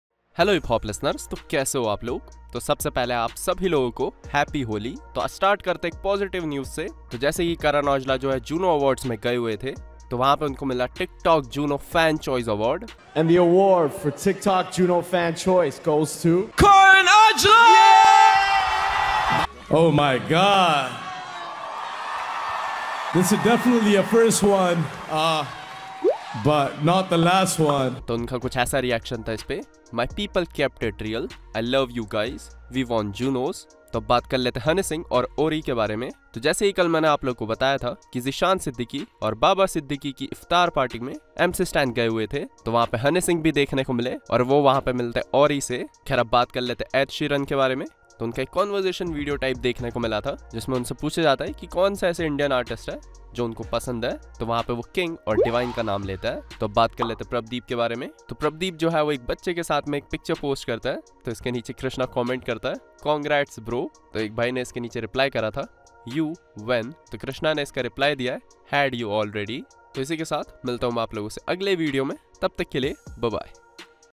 OMG😱! ED Sheeran Fav. Indian ARTIST ! Kr$na REACT😂 | Karan Aujla Juno Awards Winner! Honey with Orry
DESI/INDIAN HIP HOP UPDATES

►SONGS LINK